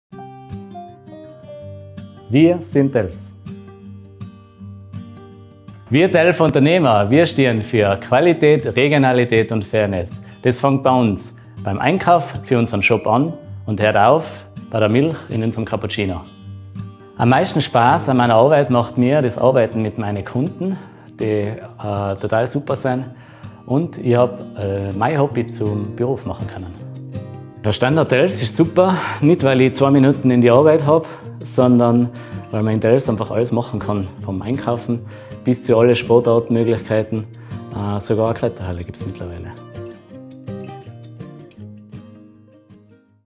wir sind TELFS - einkehren in telfs - christoph waldhart
christoph waldhart, telfer gastronom, hat sein hobby und seine passion zum beruf gemacht.

einkaufen und einkehren in telfs.